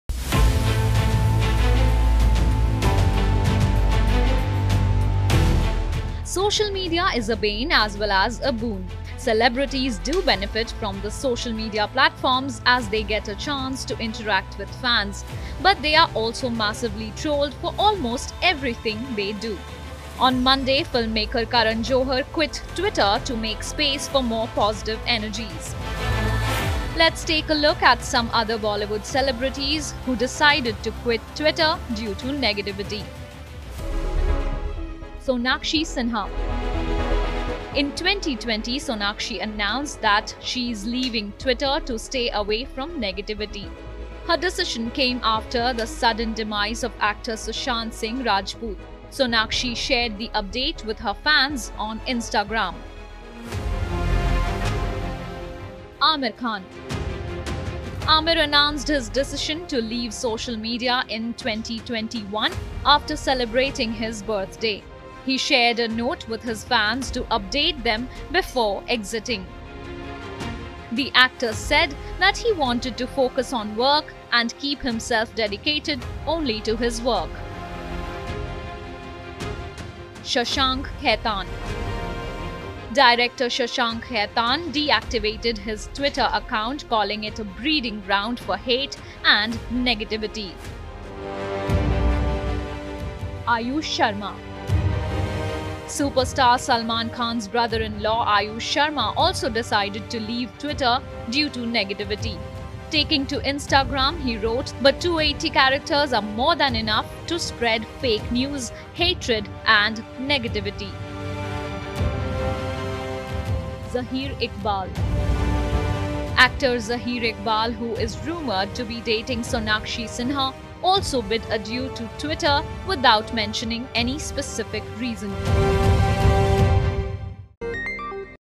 Bollywood Celebrities Who Quit Twitter
Social media is a bane as well as a boon. Celebrities do benefit from the social media platforms as they get a chance to interact with fans, but they are also massively trolled for almost everything they do. On Monday, filmmaker Karan Johar quit Twitter to make space for ‘more positive energies’. Here is a look at other celebs who quit the platform.